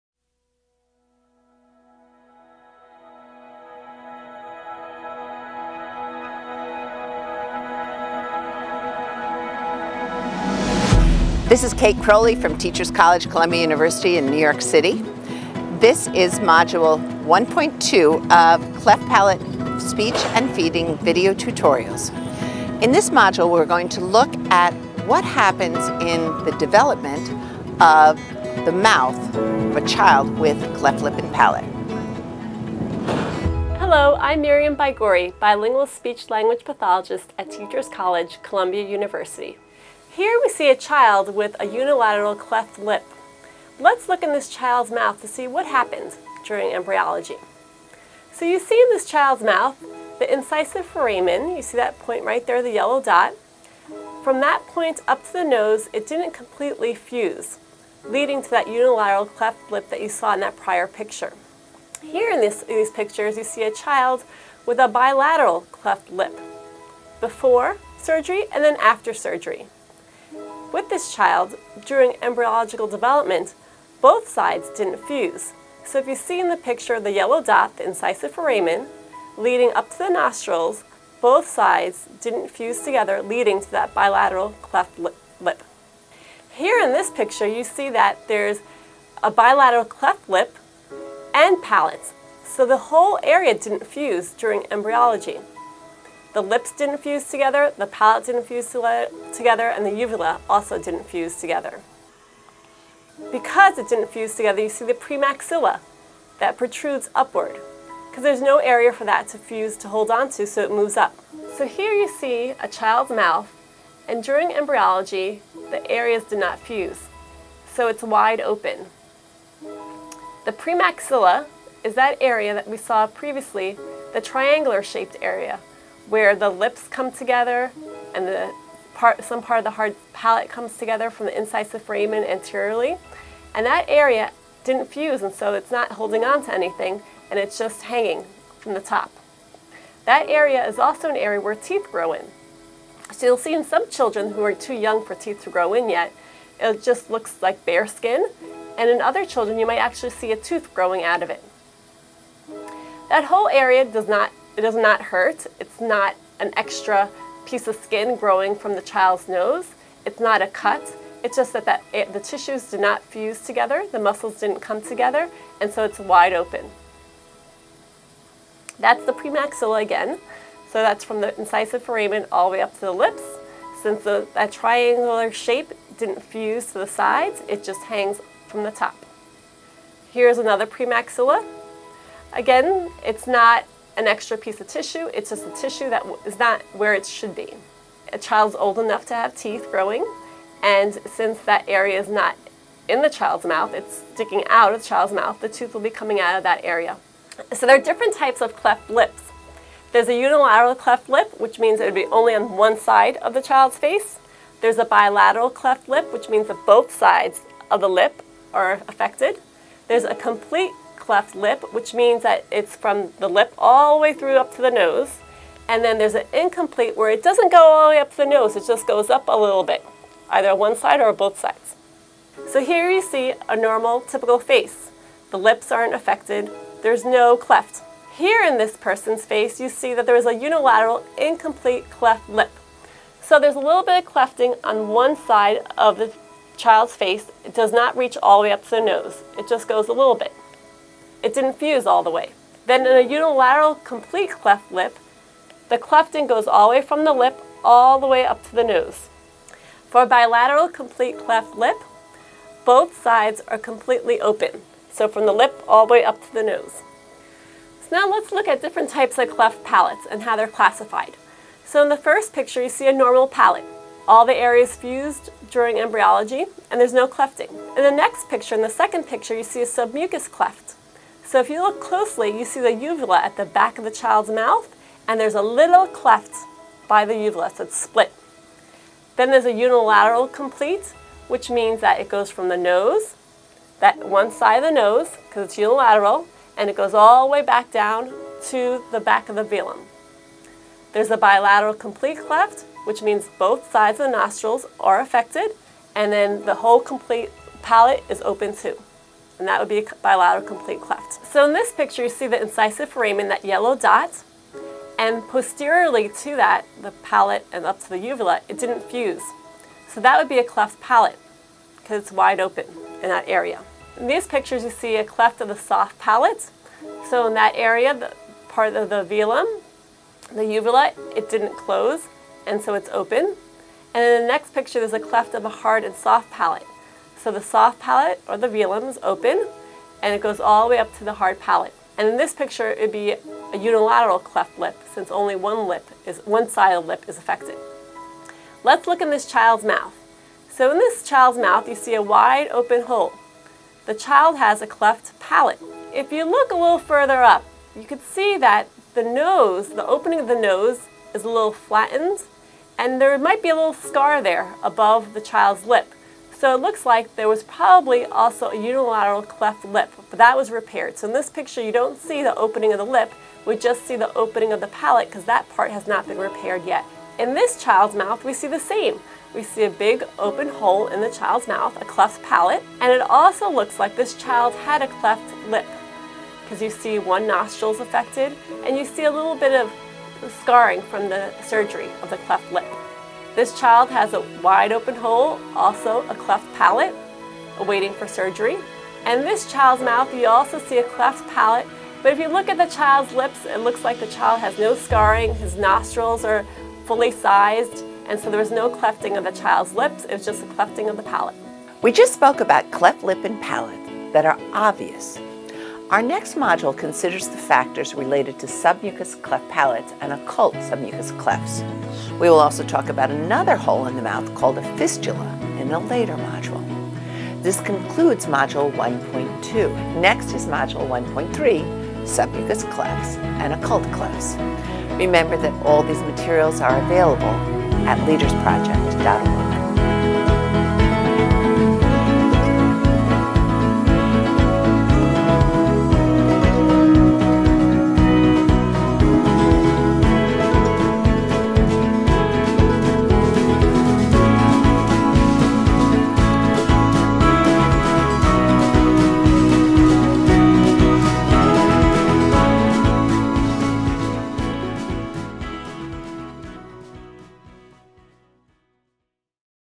Module 1.2- Cleft Palate Speech and Feeding: Anatomy and Physiology of Cleft Lip and Palate
This video is a part of a series of speech therapy and feeding tutorials to address the speech and feeding of children with cleft palate. Created by Catherine Crowley, Miriam Baigorri, and Chelsea Sommer. In Module 1.2, Cate Crowley and Miriam Baigorri discuss the anatomy and physiology of different types of cleft lip and palate.